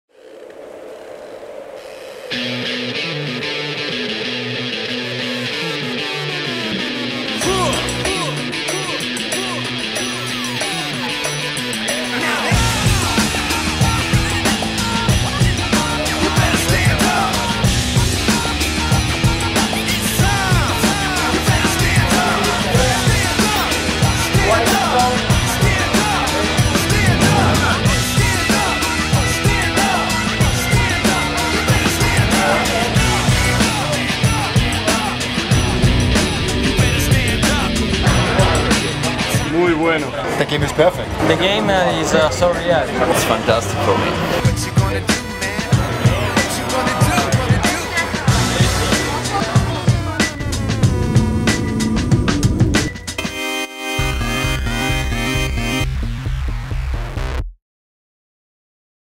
The eSport Gaming Zone
Fans now have the chance to try out the new MotoGP 17 video game in the all-new eSport Gaming Zone!

eSport Gaming Zone available at NeroGiardini Motorrad Grand Prix von Österreich, Octo British Grand Prix, Gran Premio Tribul Mastercard di San Marino e della Riviera di Rimini and Gran Premio Movistar de Aragon.